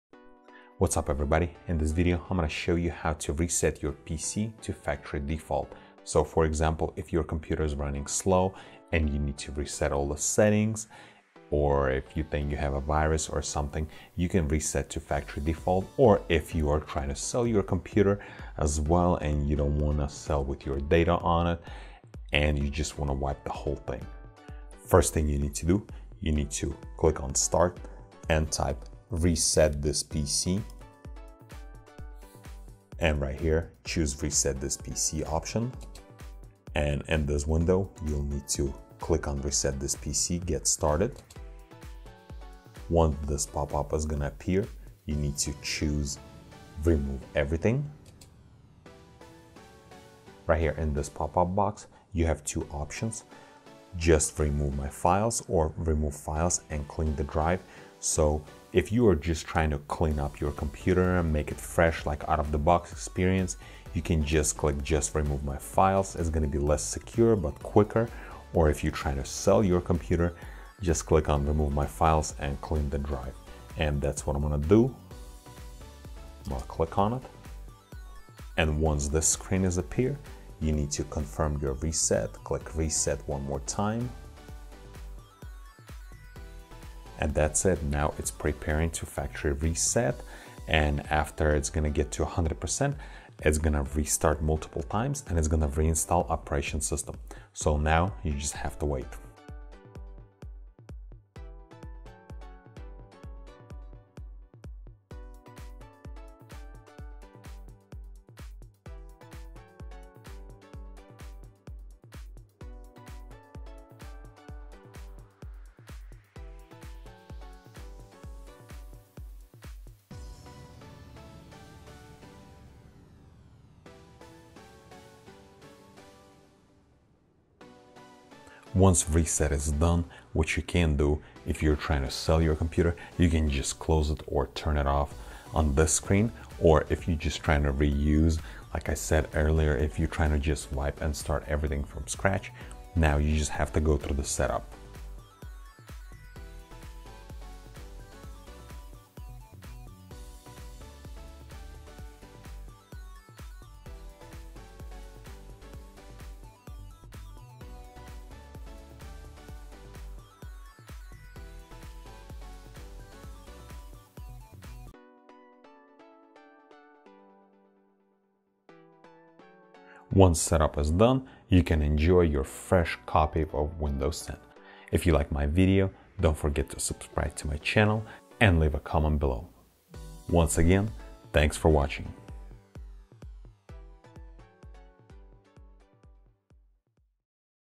How to Reset Windows 10 to Factory Settings without installation disc
Step by step on how to reset Windows 10 to factory default with installation disk, 3 minute video

PERSONAL GEAR
Main Front view camera